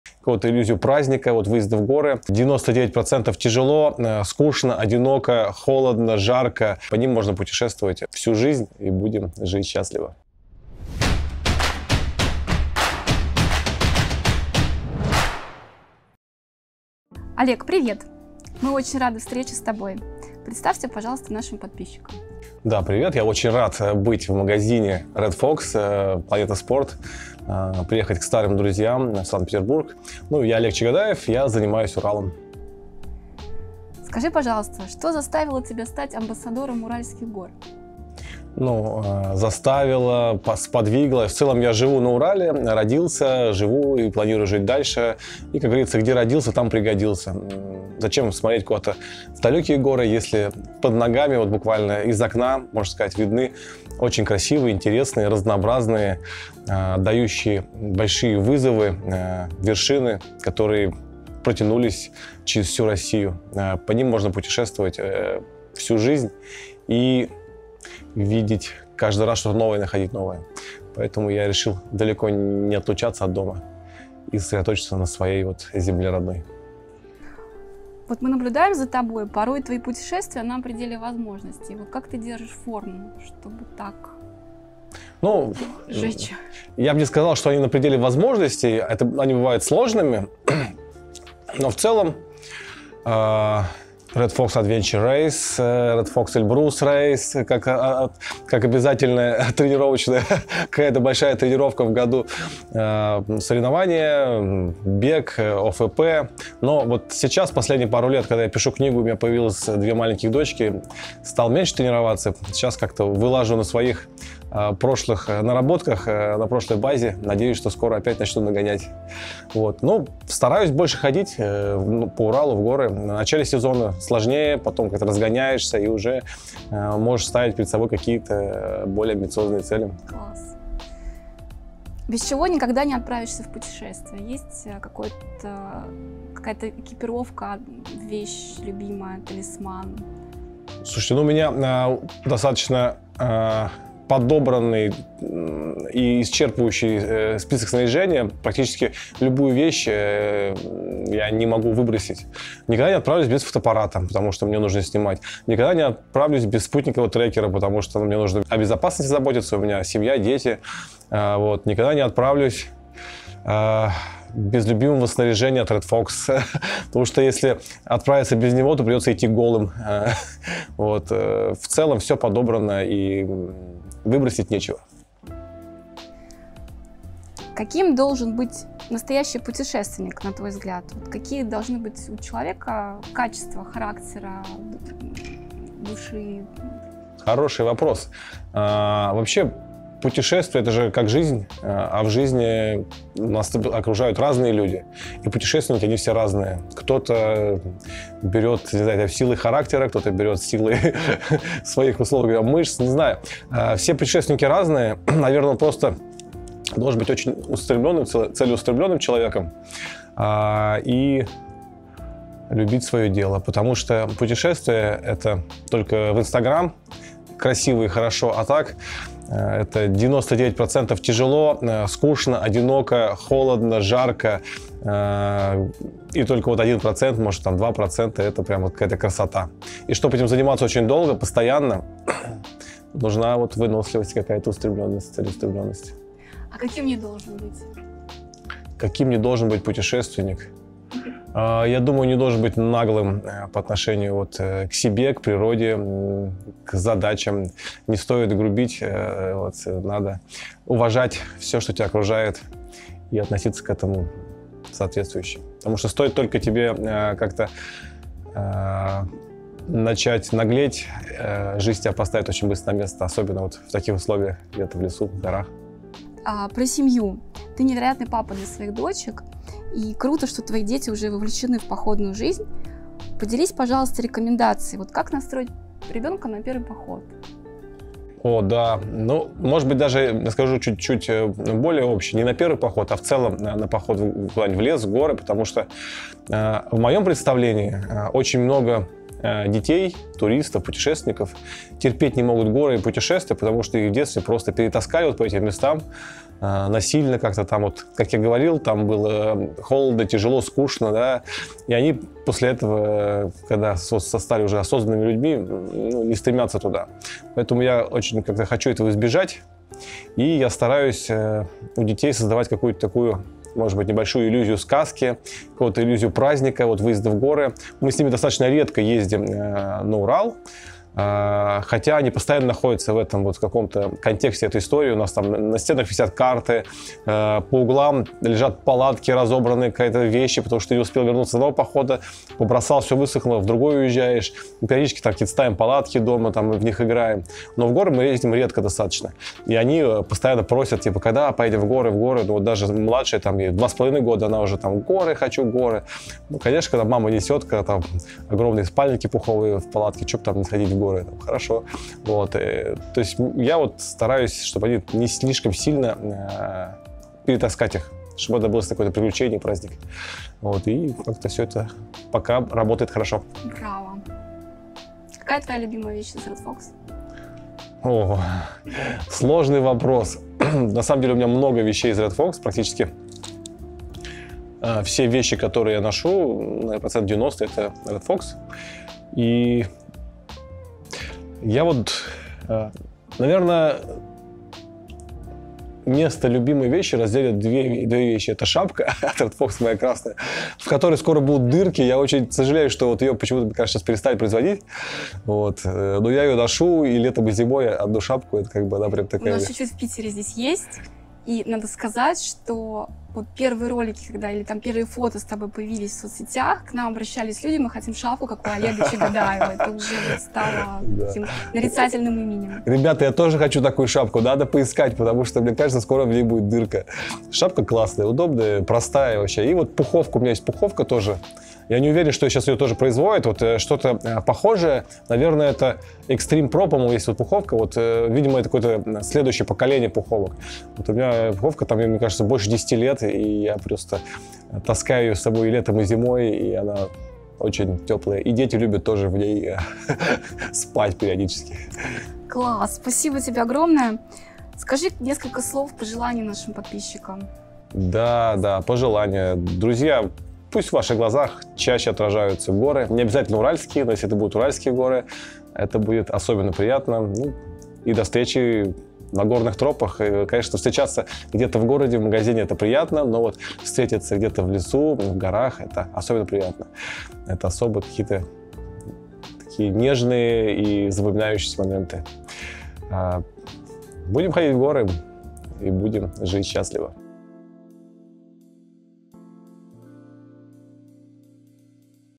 Олег Чегодаев | 9 вопросов амбассадору Уральских гор
Интервью с Олегом Чегодаевым, амбассадором Уральских гор. Про путешествия, семью, любимые горы. Смотрите, вдохновляйтесь и планируйте новые маршруты.